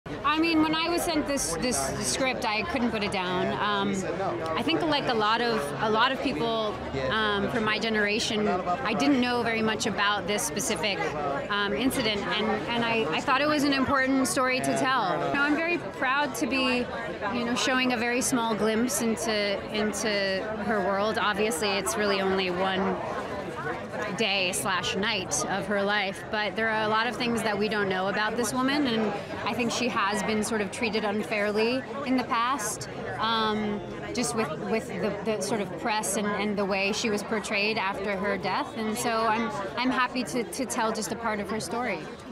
Chappaquiddick LA Premiere Red Carpet - Itw KateMara (official video)
Los Angeles, Samuel Goldwyn Theater, March 29 2018

Chappaquiddick is a 2017 American drama film directed by John Curran, and written by Taylor Allen and Andrew Logan. The film stars Jason Clarke as Ted Kennedy (U.S. Senator from Massachusetts) and Kate Mara as Mary Jo Kopechne, with Ed Helms, Bruce Dern, Jim Gaffigan, and Taylor Nichols in supporting roles. The plot details the events following the 1969 Chappaquiddick incident, wherein Senator Kennedy drove his car into a lake, killing Kopechne. The film was screened in the Gala Presentations section at the 2017 Toronto International Film Festival. The film is scheduled to be released on April 6, 2018, by Entertainment Studios.

Synopsis;
Through true accounts, documented in the inquest from the investigation in 1969, the film addresses the scandal and mysterious events surrounding the drowning of Mary Jo Kopechne (Kate Mara) after Ted Kennedy (Jason Clarke) drove his car off the infamous Dike Bridge. Not only did this event take the life of an aspiring political strategist and Kennedy insider, it ultimately changed the course of presidential history, intimately exposing the broad reach of political power, the influence of America's most celebrated family, and the vulnerability of Ted Kennedy, the youngest son, in the shadow of his family legacy.

Chappaquiddick
Directed by John Curran
Produced by Mark Ciardi, Chris Cowles, Campbell G. McInnes
Written by Taylor Allen, Andrew Logan
Starring Jason Clarke, Kate Mara, Ed Helms, Bruce Dern, Jim Gaffigan, Taylor Nichols
Cinematography : Maryse Alberti
Edited by Keith Fraase
Production company : Apex Entertainment
Distributed by Entertainment Studios Motion Pictures
Release date : September 10, 2017 (TIFF), April 6, 2018 (United States)
Running time : 101 minutes

(Source: Wikipedia)